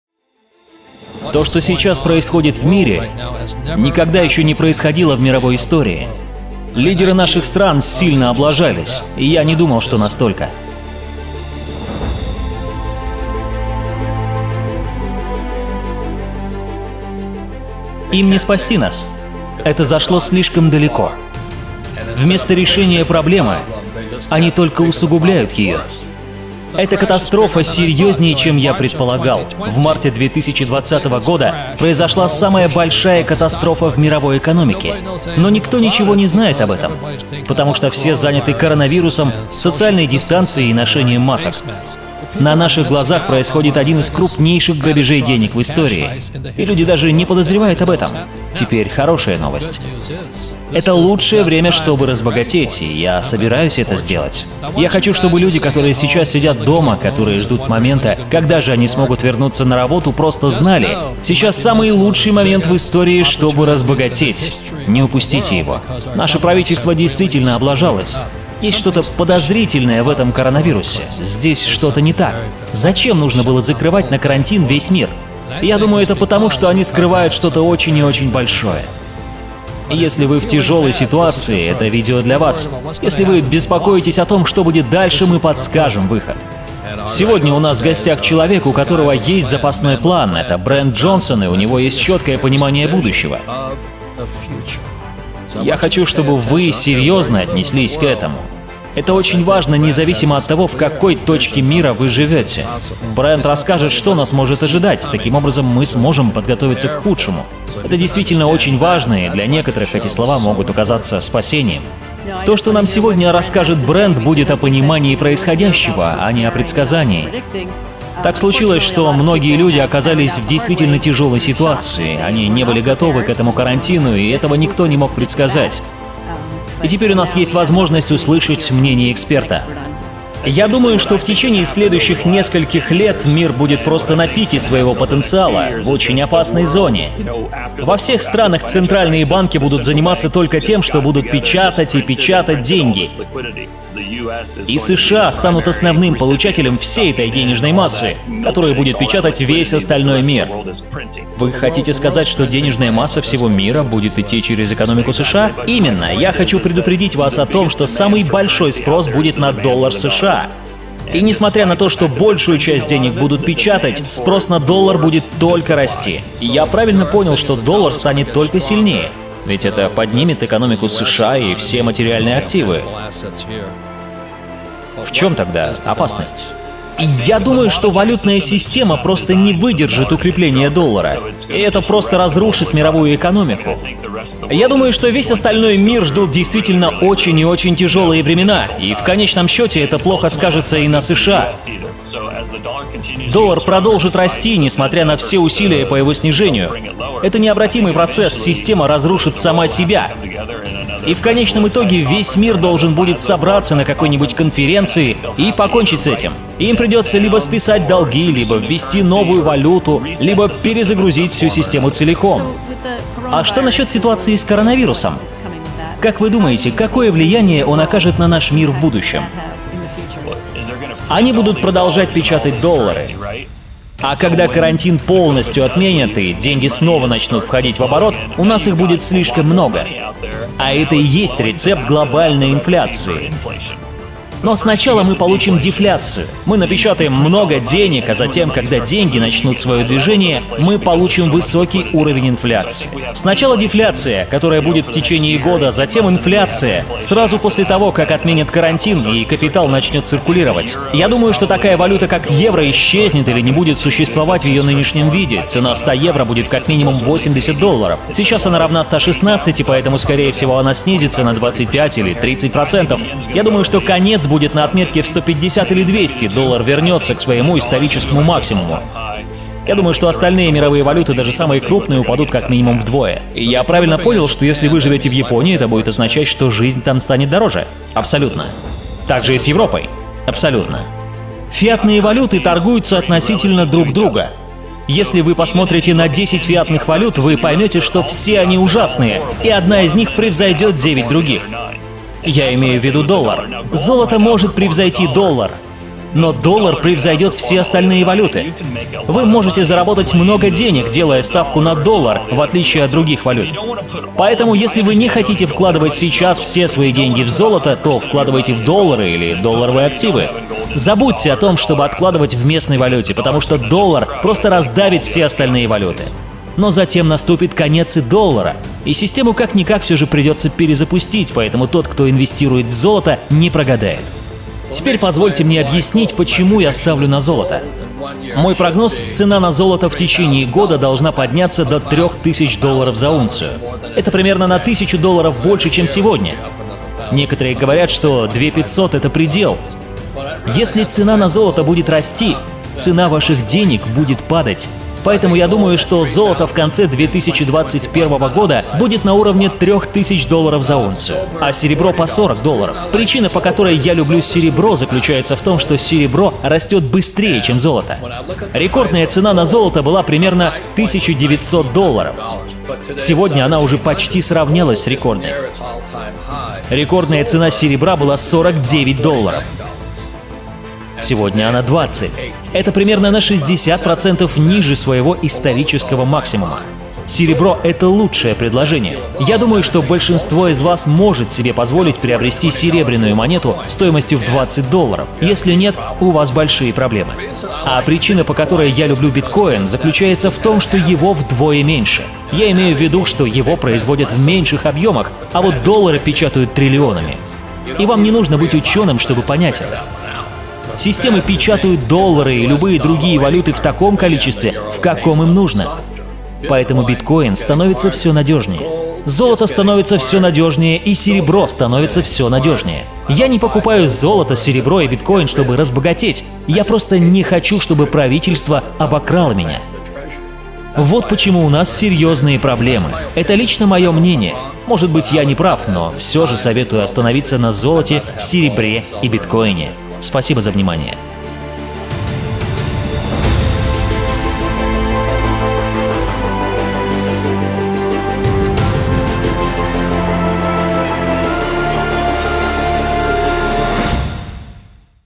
Роберт Кийосаки - Готовьтесь к Худшему! ЭТО ОЧЕНЬ СЕРЬЕЗНО! Мир уничтожит сам себя!
Роберт Кийосаки - Мир на Грани! Что нас ждет после карантина?